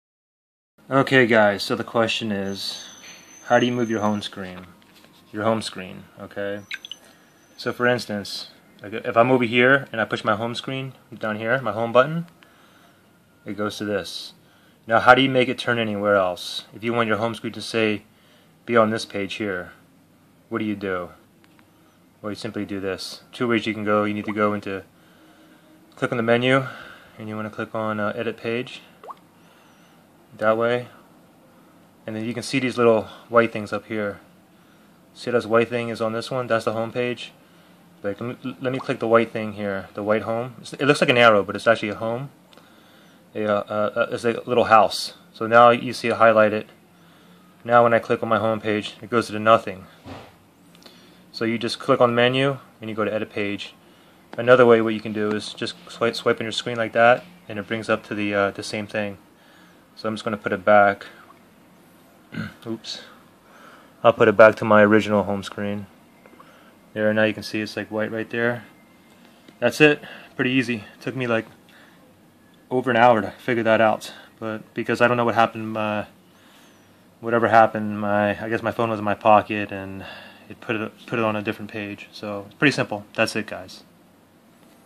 How to Move your Home Screen: Galaxy S4
How to Move (or place back to default) your Home Screen on a Samsung Galaxy S4 / Note